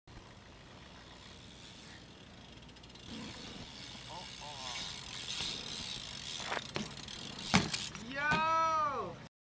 Tucson Bike Shop- Local Craycroft Cycles rider, Jesse Erickson
Jesse Erickson riding for Craycroft Cycles